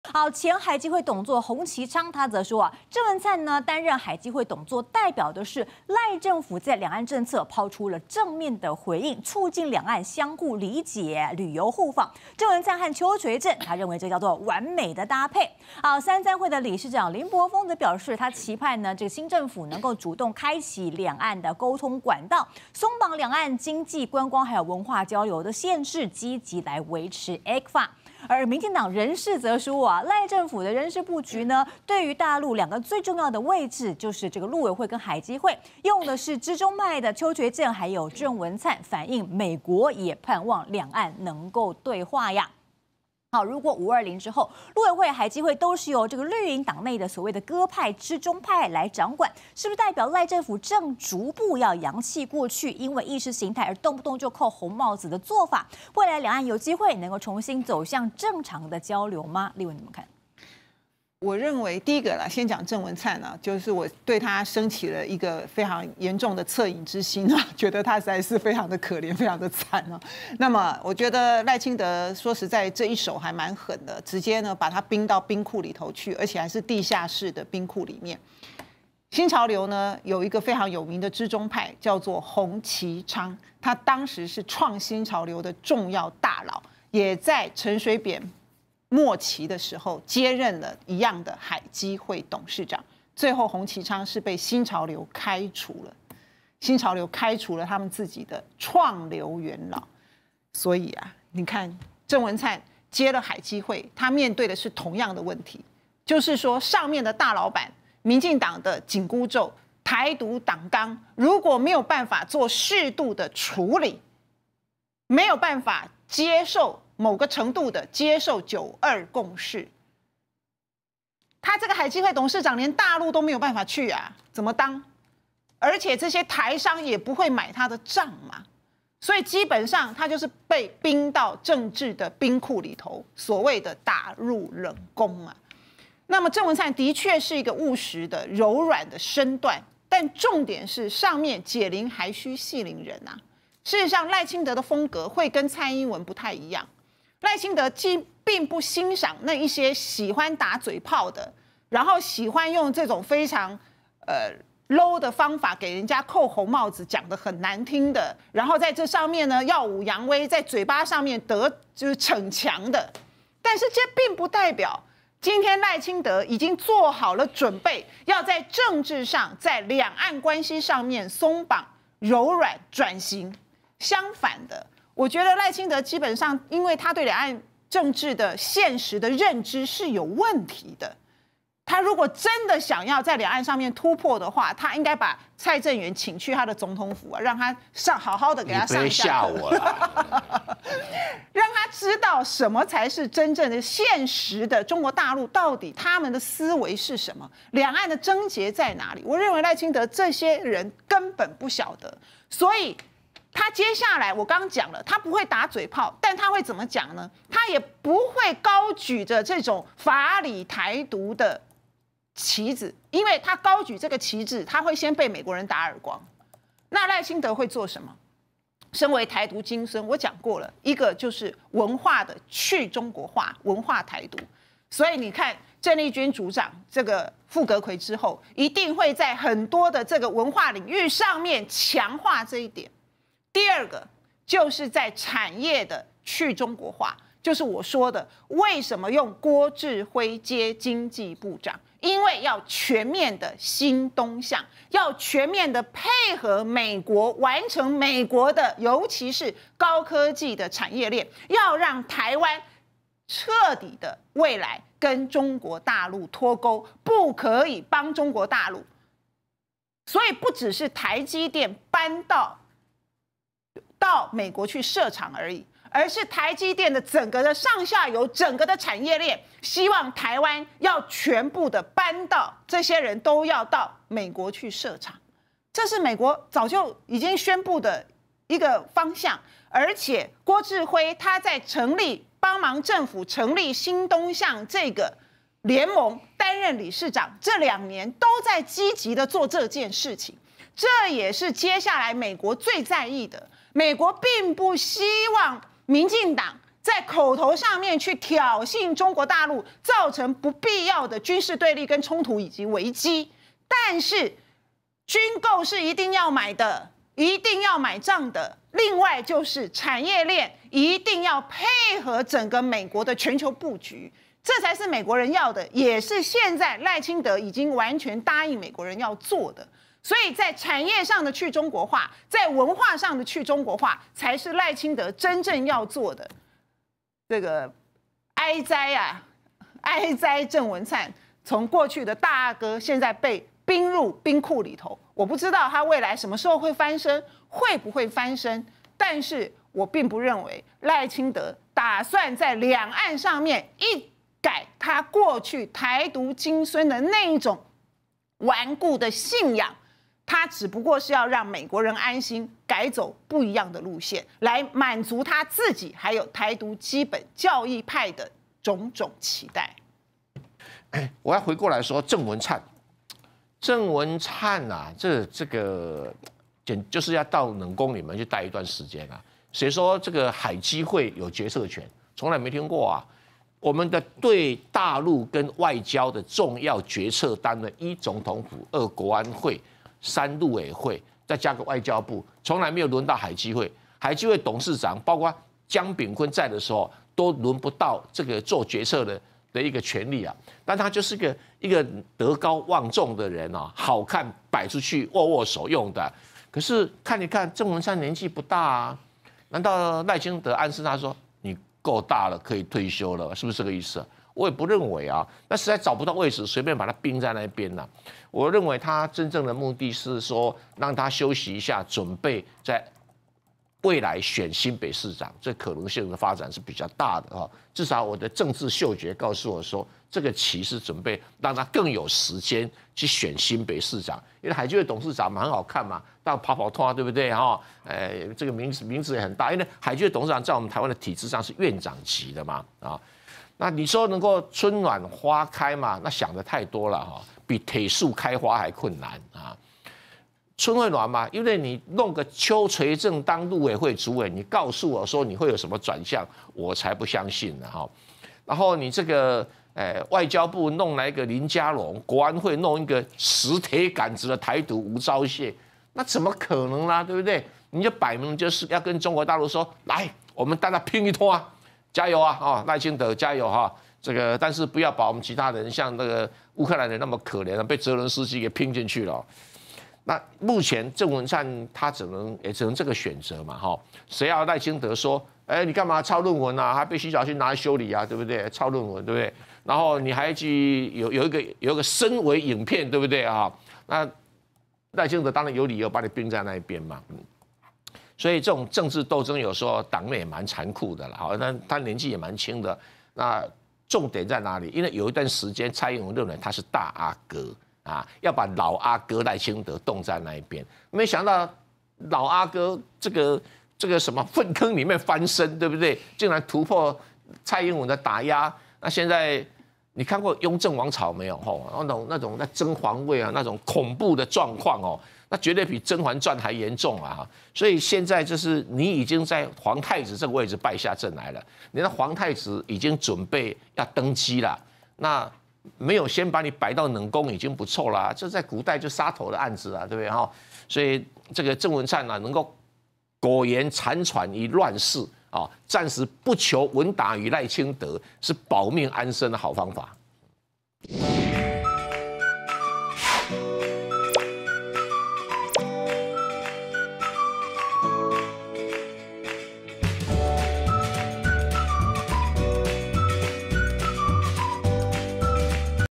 知中派鄭文燦掌海基會 蔡正元嗅到賴清德背後布局 鄭麗文:解鈴還須繫鈴人
責任小編:wei編/很會編